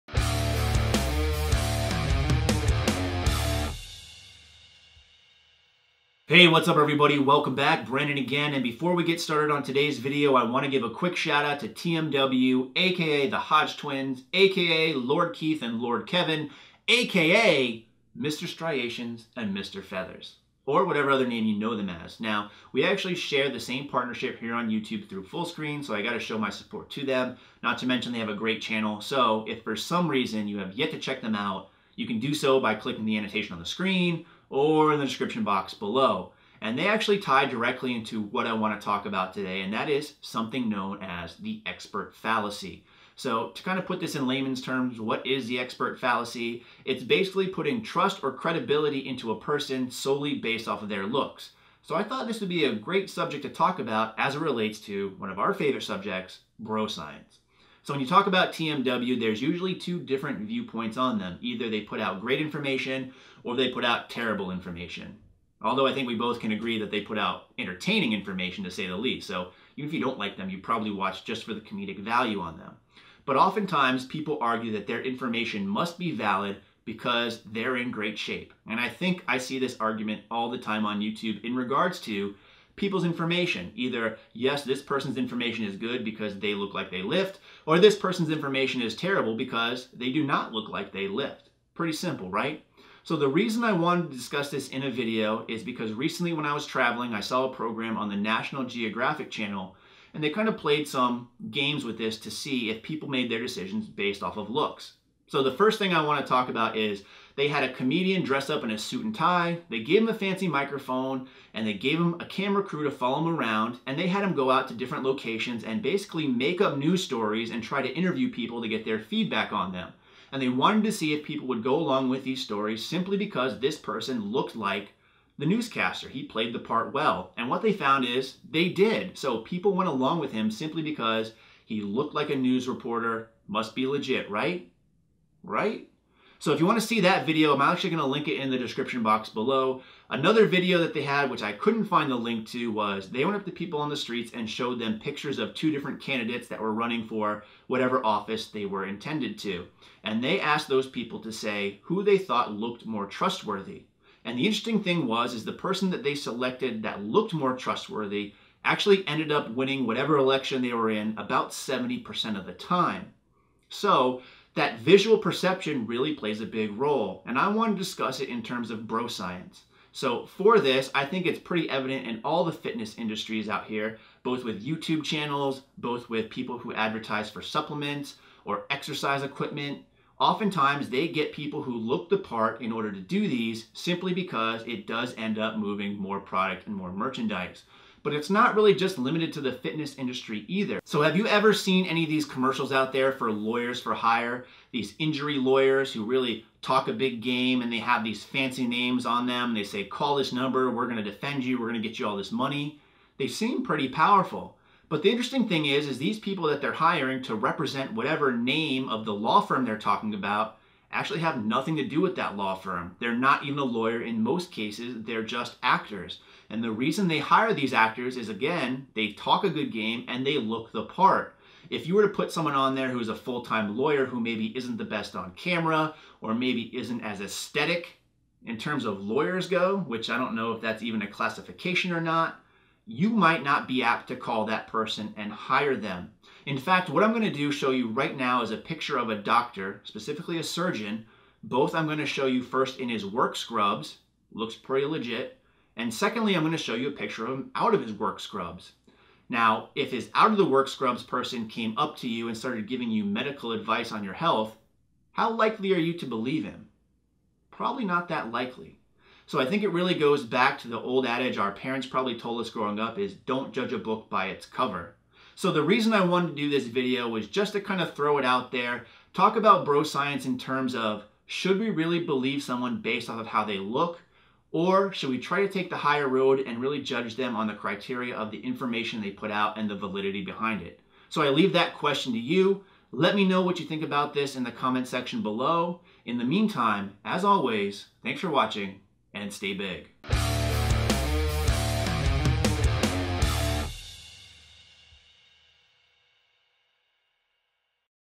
Hodge Twins - Bro Science - Expert Fallacy
Do we believe everything we hear? What about everything we see? What if what we see influences how we process what we hear? In this video we broach the subject of something known as the expert fallacy. Leave your feedback below whether or not you think this holds true.